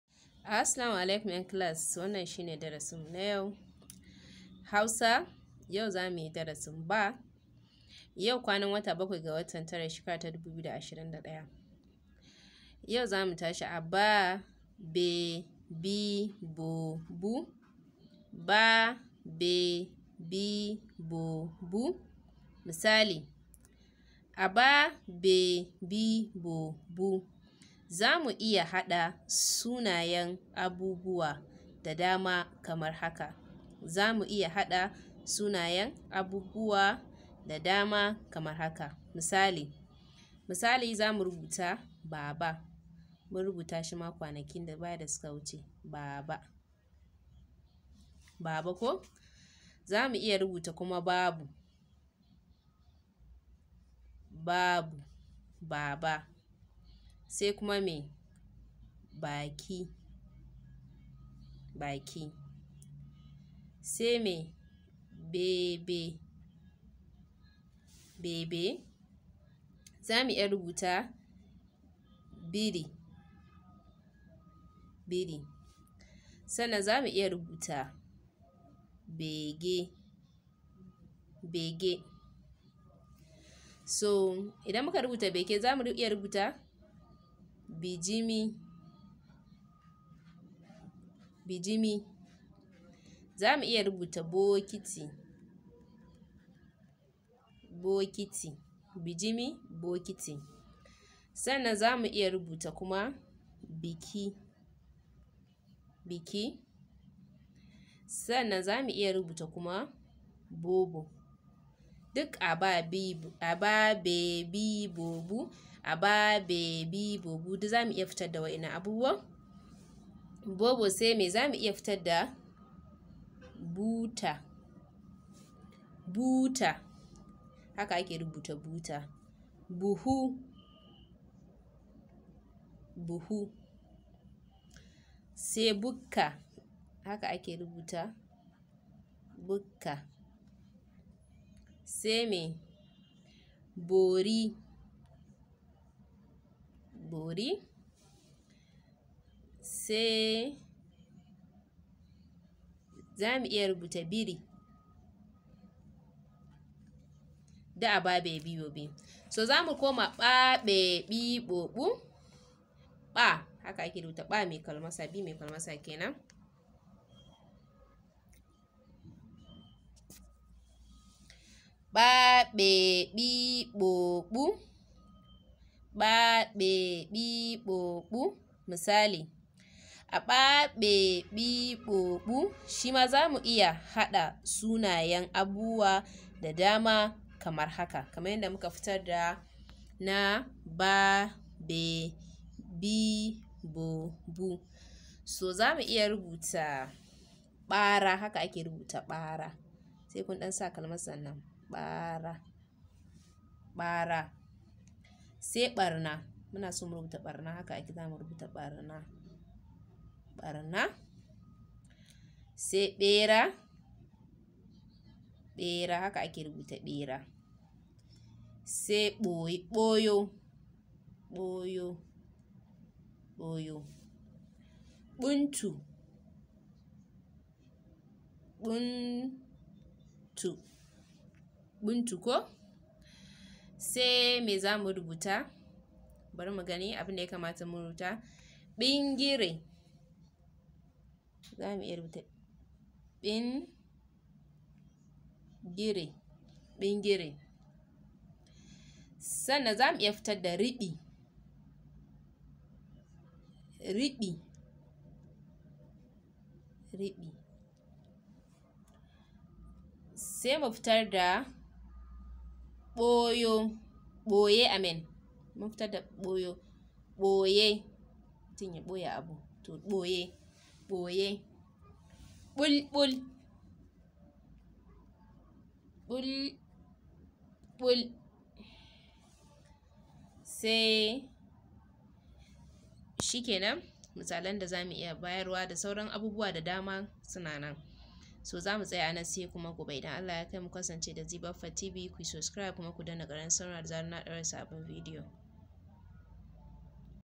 yadda zaku koyi iya rubutun hausa suna ,abubuwa, da sauransu karantawa zeebappahtv in 2021
published on 7/9/2021 wanna video yana magana ne akan yadda zaku koyi rubuta sunayen mutane da abubuwa da sauransu